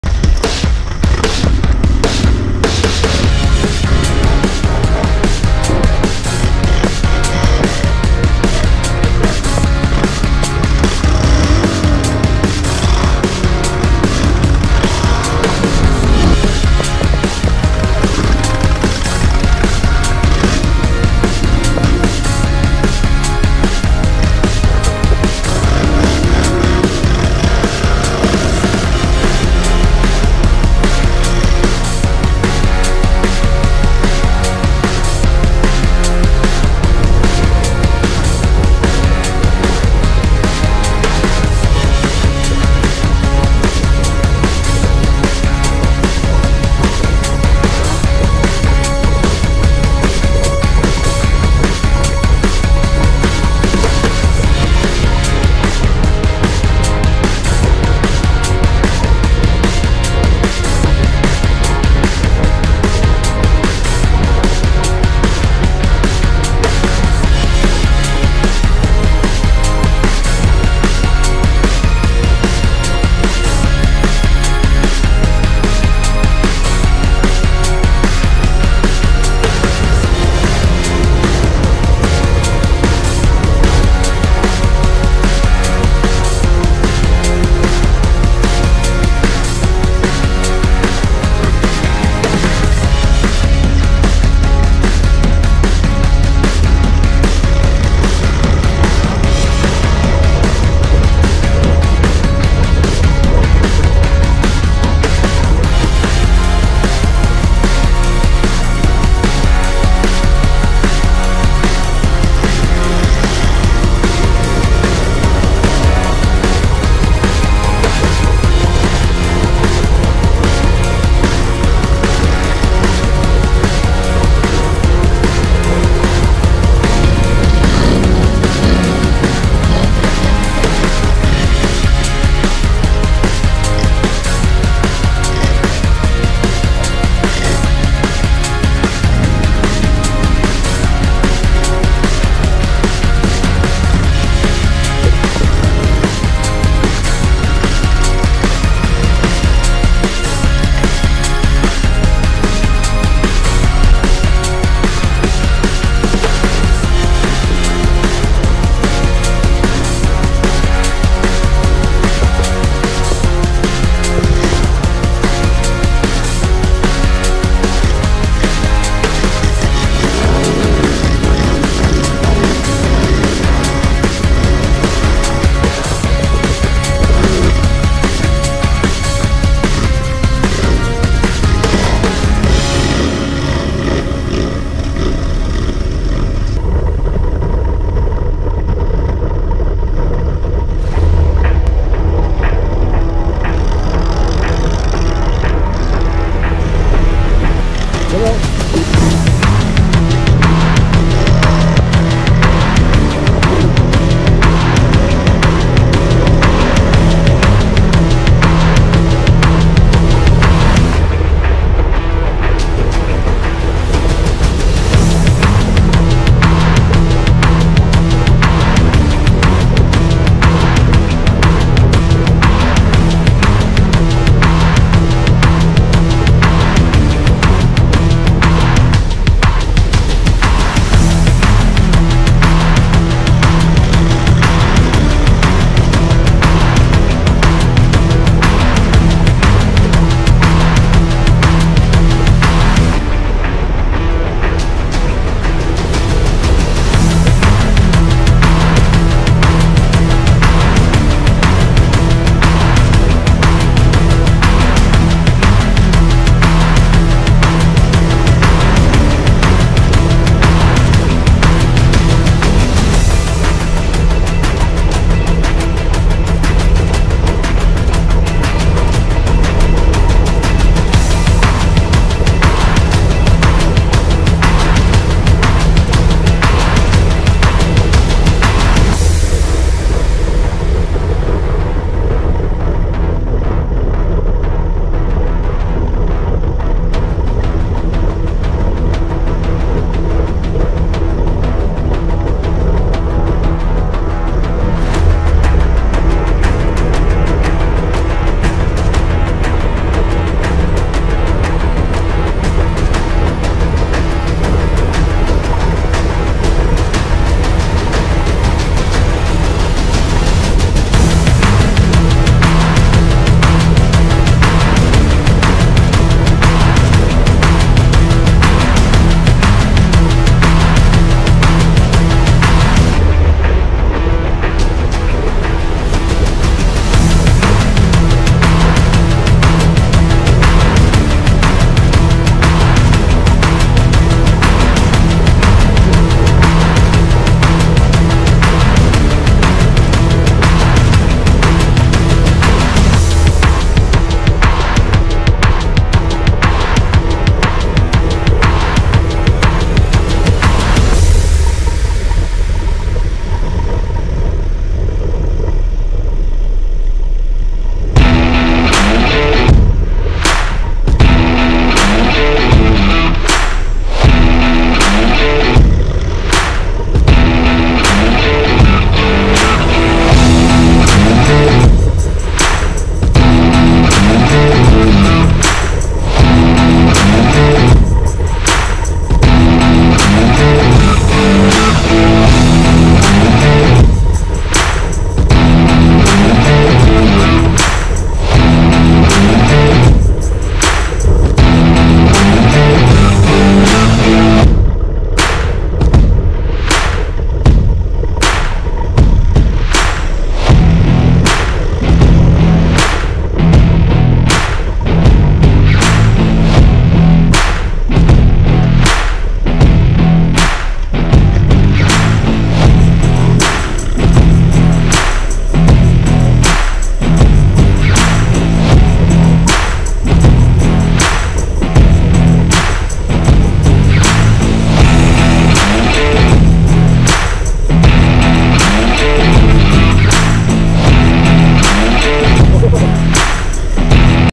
run et picnique klosk dura et dark fenrir
Petite sortie (2h) entre les 2 asso, les KLOSK-DURA et les DARK-FENRIR